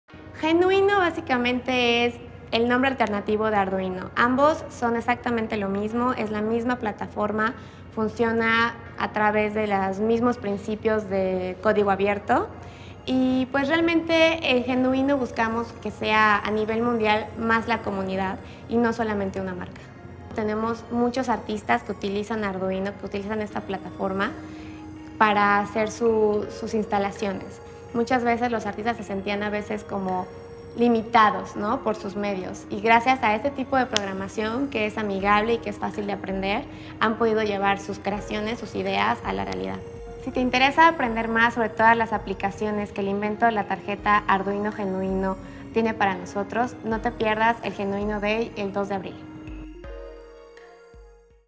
¿Qué es la Tarjeta Genuino?
Genuino, el nombre alternativo de Arduino, es una plataforma que funciona a través de código abierto y que gracias a su accesibilidad ha sido adoptada por artistas conceptuales lo mismo que por gente que no tiene estudios de robótica e ingeniería con algunos resultados asombrosos.